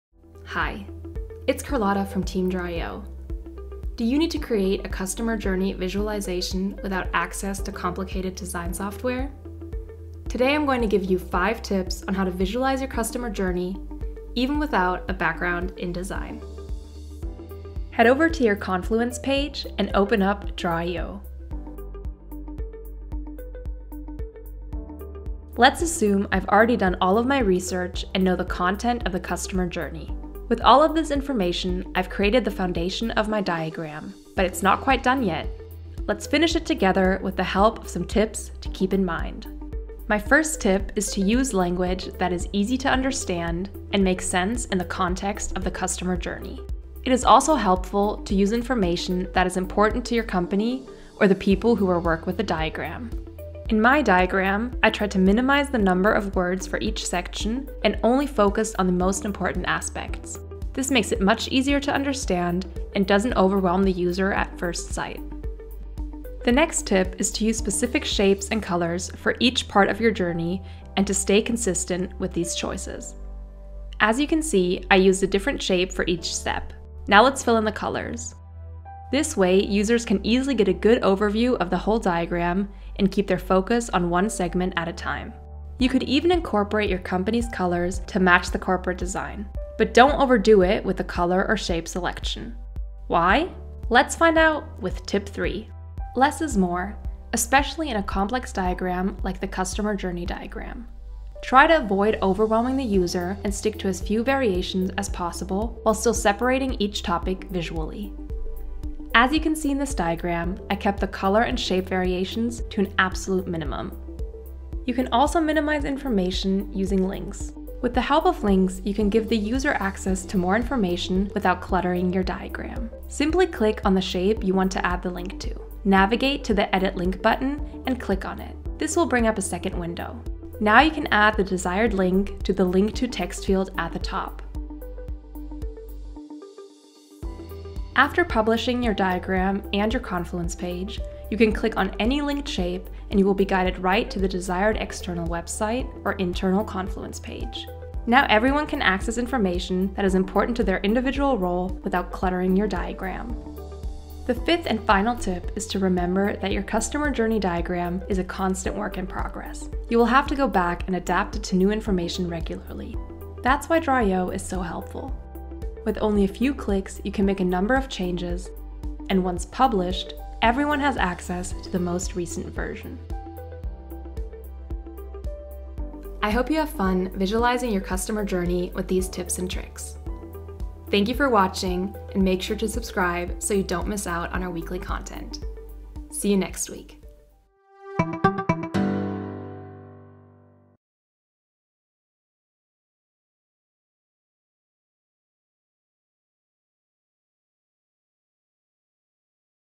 Five Tips on How to Visualize Your Customer Journey in draw.io
If you want to easily visualize customer journeys in draw.io, keep watching to receive five helpful tips to support your diagramming process.